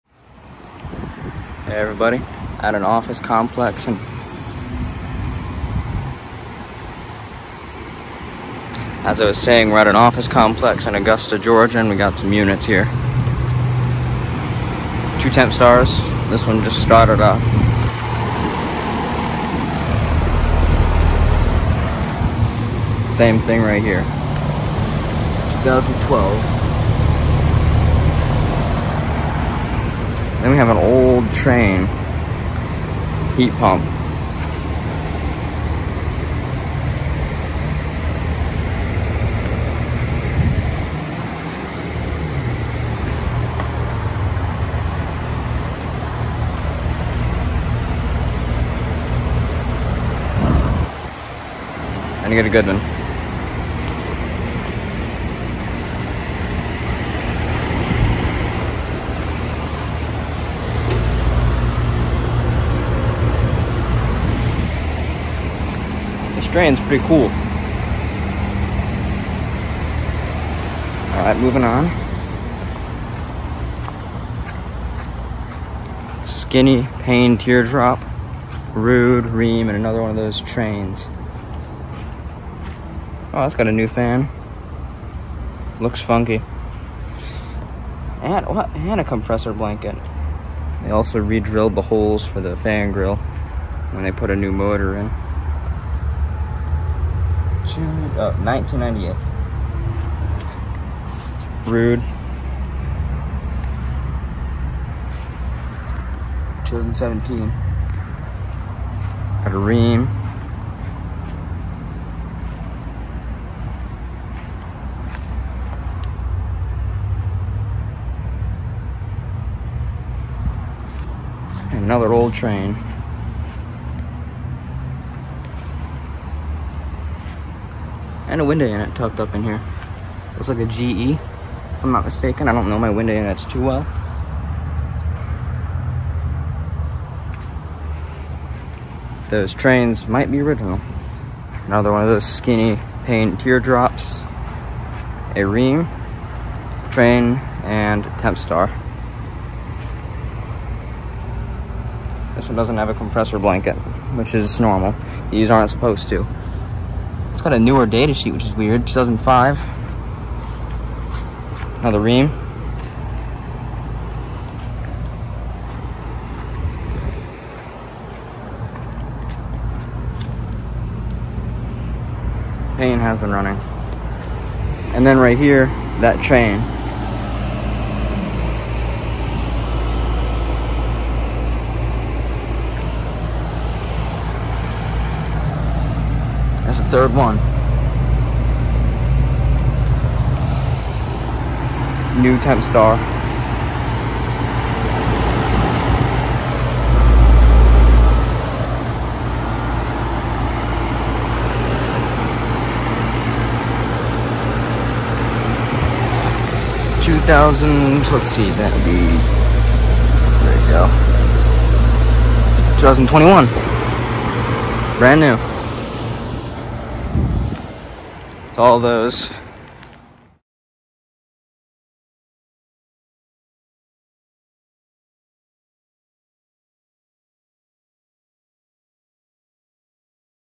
HVAC Heaven Part 1 - Tempstar, Rheem, Trane Heat Pumps and more
A bunch of units, located in a huge business complex in Augusta, GA. This is part one of a 4 part series.

Time-stamps:
0:00 - Intro
0:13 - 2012 Tempstar Heat Pumps
0:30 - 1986 Trane Executive Heat Pump
0:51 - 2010 Goodman Heat Pump
1:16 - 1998 Payne Teardrop Heat Pump
1:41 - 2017 Ruud Achiever Heat Pump
1:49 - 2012 Rheem Classic Heat Pump
2:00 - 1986 Trane Executive Heat Pump
2:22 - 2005 Payne Teardrop Heat Pump
2:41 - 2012 Rheem Classic Heat Pump
2:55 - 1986 Trane Executive Heat Pump
3:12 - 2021 Tempstar Heat Pump
3:38 - Outro